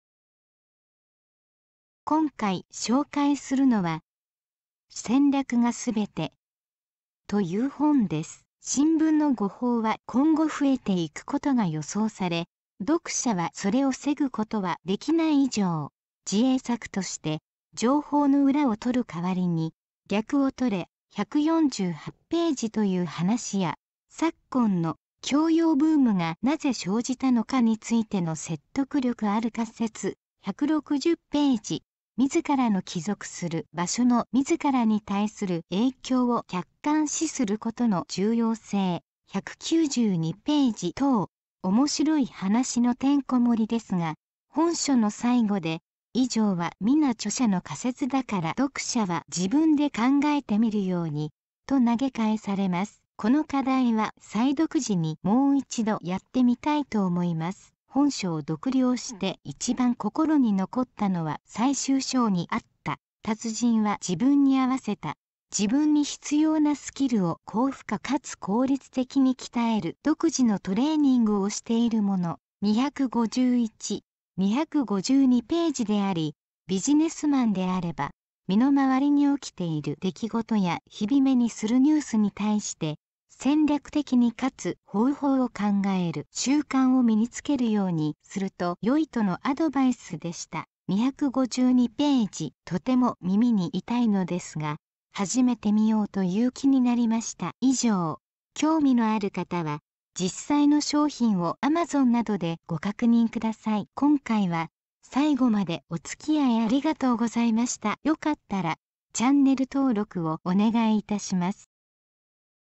【紹介】戦略がすべて 新潮新書 （瀧本 哲史）
今回紹介するのは、　戦略がすべて　という本です。

新聞の誤報は今後増えていくことが予想され、読者はそれを防ぐことはできない以上、自衛策として、情報の裏をとる代わりに、逆をとれ（148頁）という話や、昨今の「教養ブーム」がなぜ生じたのかについての説得力ある仮説（160頁）、自らの帰属する場所の自らに対する影響を客観視することの重要性（192頁）等、面白い話のてんこ盛りですが、本書の最後で、以上はみな著者の仮説だから読者は自分で考えてみるように、と投げ返されます。この課題は再読時にもう一度やってみたいと思います。

本書を読了して一番心に残ったのは最終章にあった「達人は自分に合わせた、自分に必要なスキルを高負荷かつ効率的に鍛える独自のトレーニングをしているもの」（251、252頁）であり、ビジネスマンであれば、「身の回りに起きている出来事や日々目にするニュースに対して、戦略的に勝つ方法を考える習慣を身につける」ようにすると良いとのアドバイスでした（252頁）。とても耳に痛いのですが、始めてみようという気になりました。